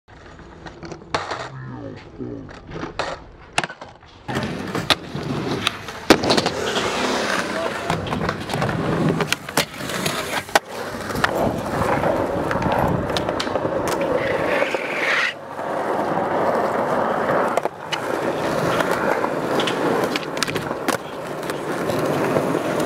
Clip of the Day - Myles Wright
filmed at birmingham skatepark with a canon 60D.